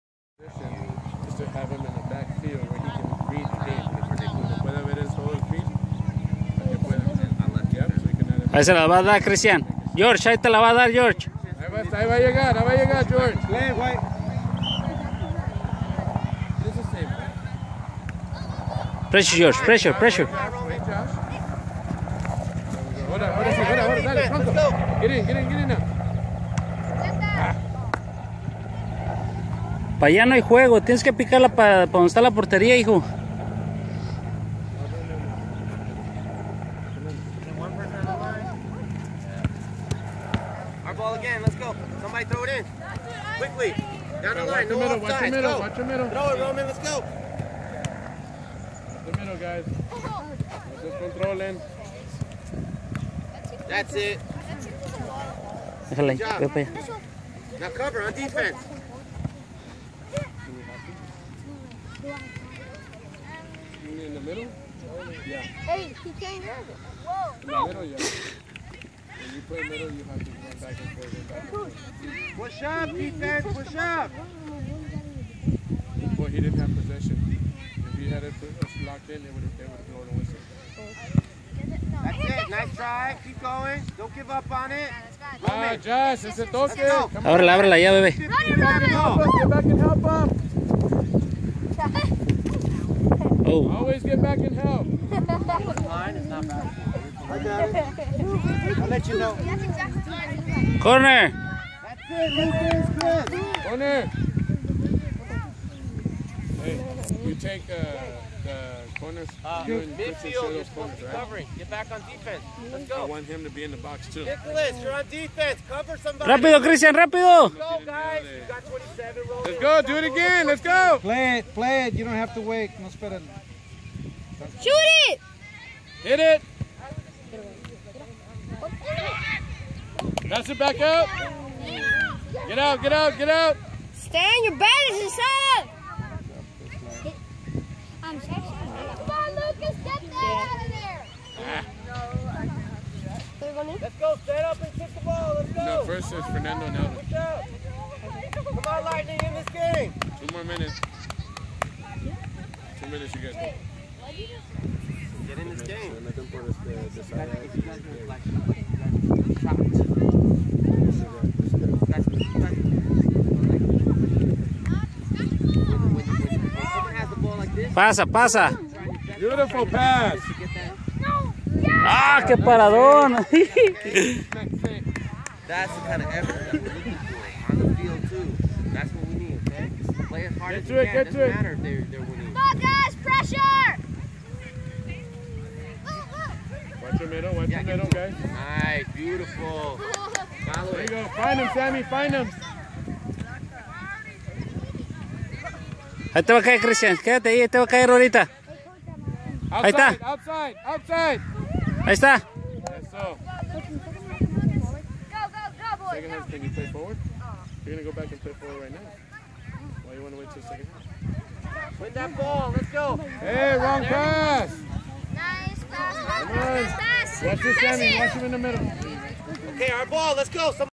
Christian Casillas Soccer Skills #
Club Northstars U9 VS Berkley Ca at the San Lorenzo Park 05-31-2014